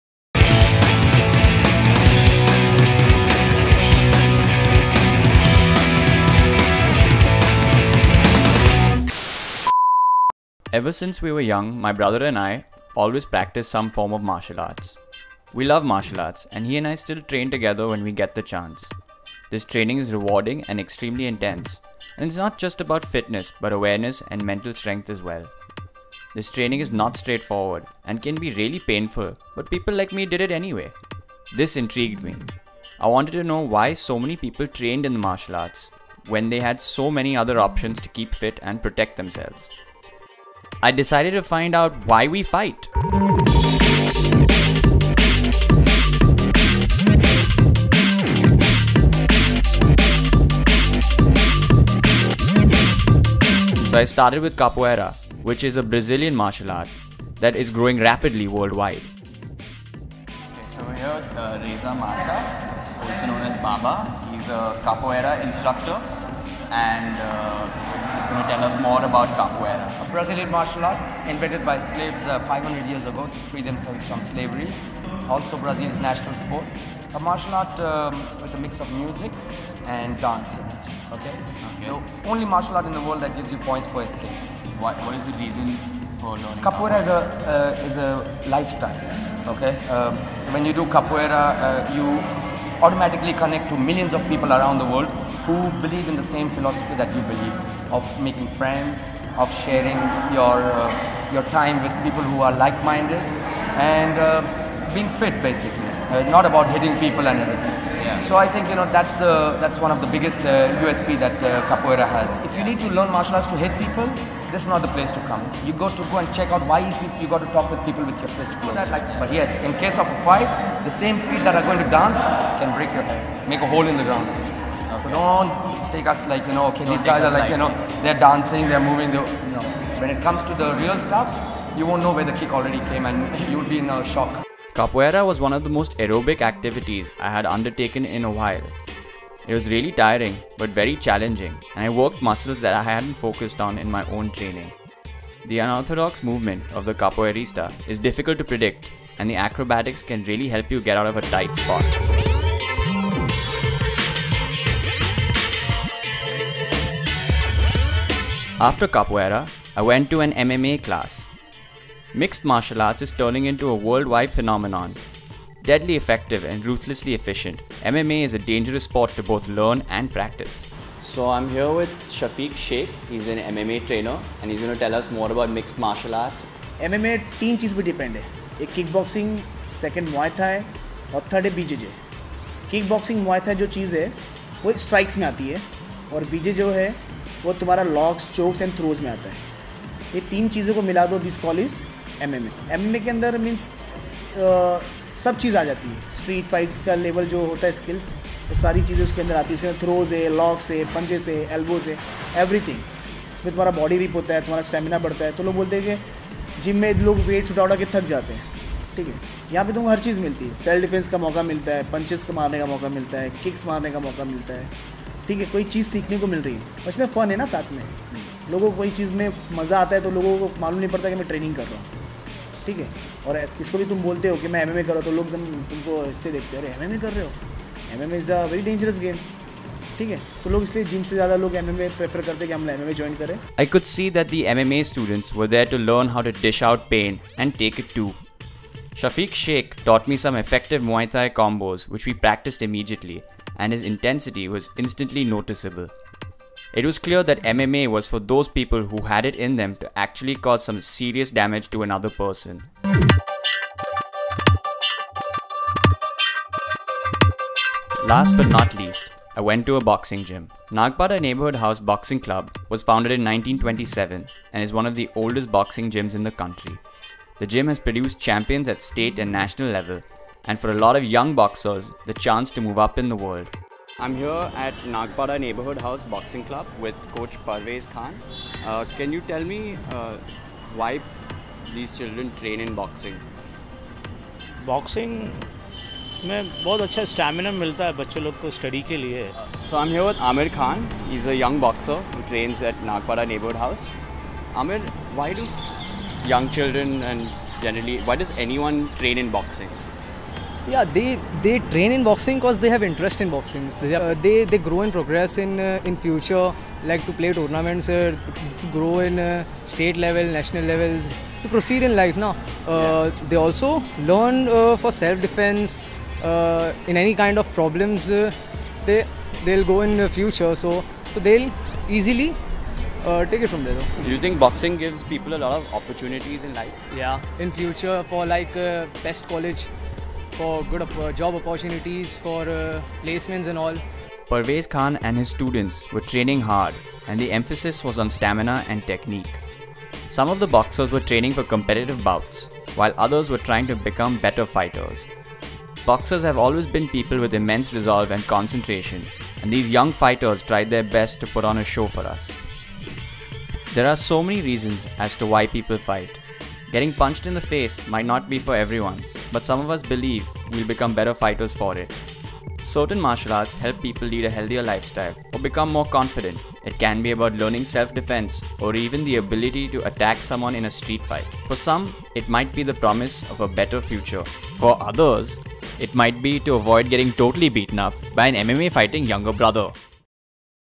Why We Fight
Join NH7 Features Writer Siddhant Mehta as he trains in Capoeira, MMA and Boxing to find out why people learn these martial arts.